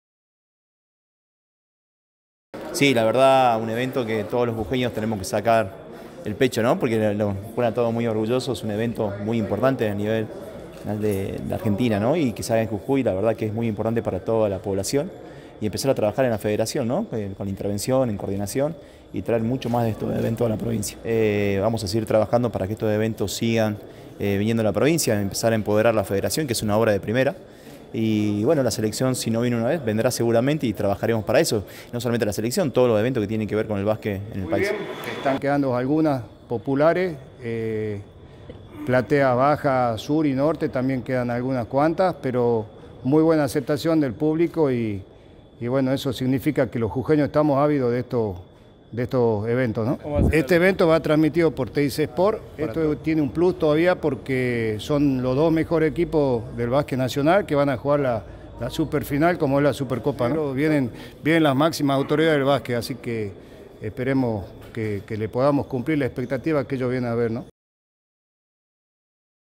Operativo de seguridad por la final de la Supercopa de la Liga Nacional de Basquet en Jujuy.
El secretario de Deportes de la provincia, Luis Calvetti, y el interventor de la Federacion Jujeña de Basquet, Gustavo Lamas, participaron de la planificación y simulacro del operativo de seguridad a desplegarse en el partido entre Quimsa de Santiago del Estero y Gimnasia de Comodoro, por la final de la Supercopa de la Liga Nacional de Basquet que se disputará el próximo.miércoles en Jujuy.